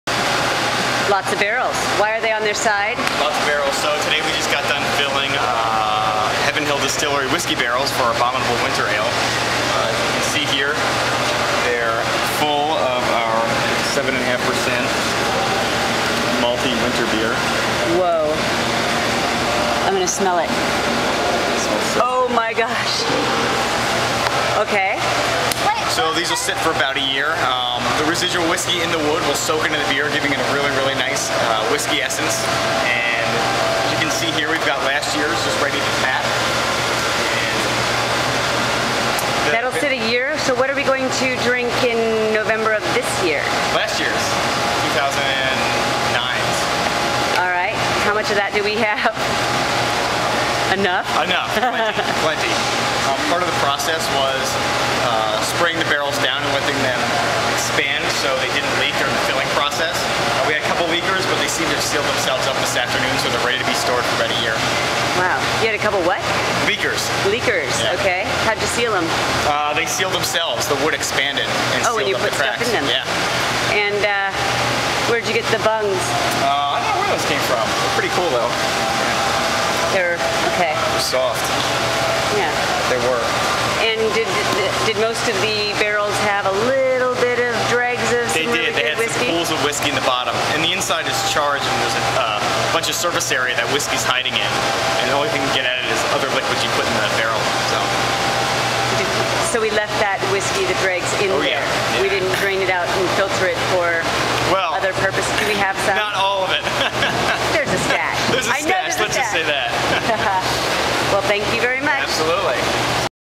Barreling Abominable
Brewer Casey Gish talks about putting our Abominable Winter Ale into Heaven Hills whiskey barrels.  The beer will mingle with the traces of 18-year old bourbon for up to a year to produce our legendary Bourbon Abominable or "B-Bomb" for short.